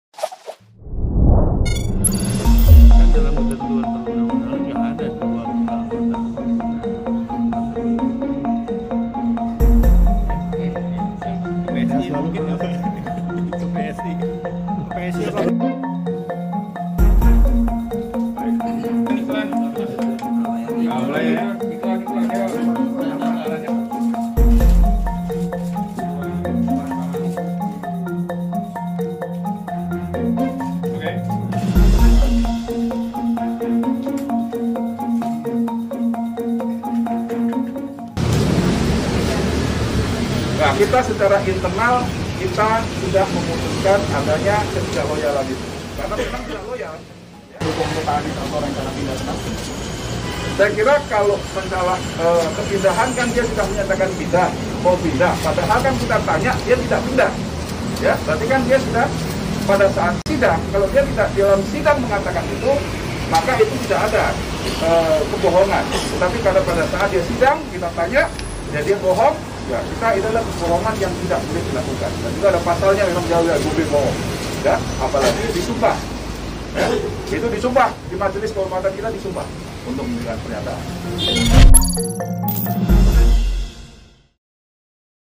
Gerindra Tegaskan Pemecatan M Taufik Tak Berkaitan Dengan Anies
Wakil Ketua Mahkamah Partai Gerindra, Wihadi Wiyanto menegaskan pemecatan Muhammad Taufik tak terkait dengan Gubernur DKI, Anies Baswedan. Ia menyebut pemecatan murni karena permasalahan loyalitas.